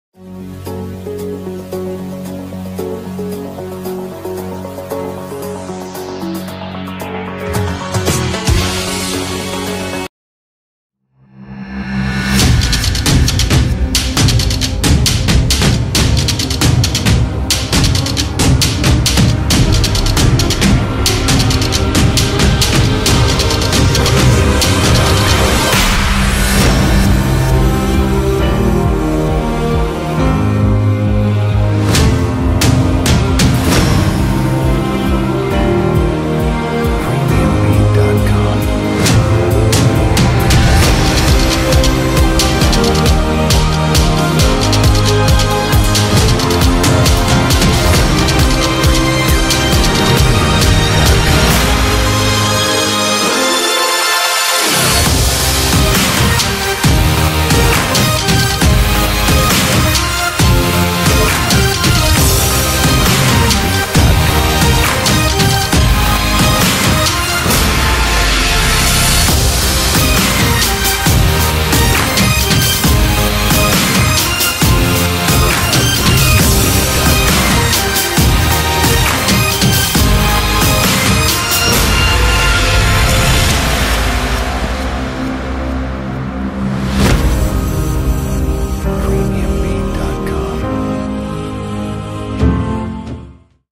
Intervencija | Voda teče v hišo, Zvirče 27.12. 2017
Dne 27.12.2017, ob 17.50 smo dobili poziv, da voda z magistrale priteka in zaliva dvorišče ter ogroža hišo. Na kraj smo odšli z GVV-2 in GVM-1, kjer smo s 16 člani namestili protipoplavne vreče, očistili jaške in izkopali jarke, s katerimi smo zagotovili odvajanje vode.